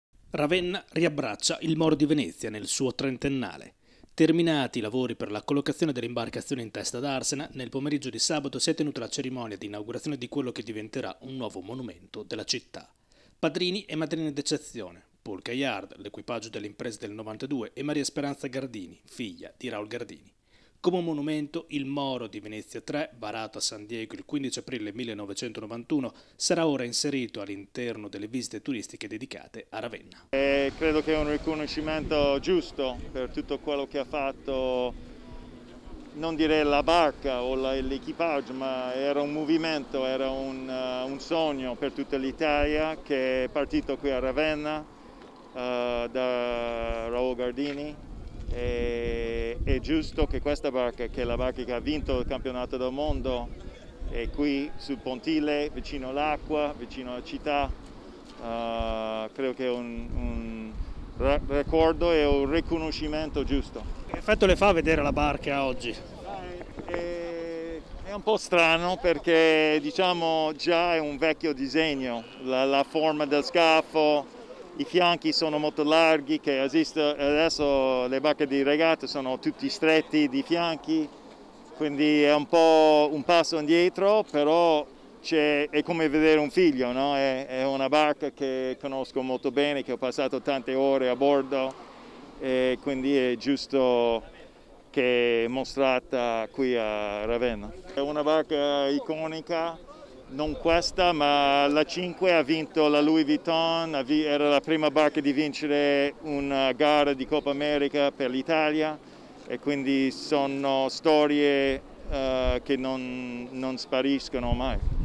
Ravenna riabbraccia Il Moro di Venezia. Paul Cayard: "È come un figlio"
Ravenna riabbraccia Il Moro di Venezia nel suo trentennale. Terminati i lavori per la collocazione dell’imbarcazione in testa Darsena, nel pomeriggio di sabato si è tenuta la cerimonia d’inaugurazione di quello che diventerà un nuovo monumento ravennate. Padrini e madrine d’eccezione: Paul Cayard, l’equipaggio delle imprese del 1992 e Maria Speranza Gardini, figlia di Raul Gardini. Come un monumento, il Moro di Venezia III, varato a San Diego il 15 aprile 1991, sarà ora inserito all’interno delle visite turistiche dedicate alla città di Ravenna.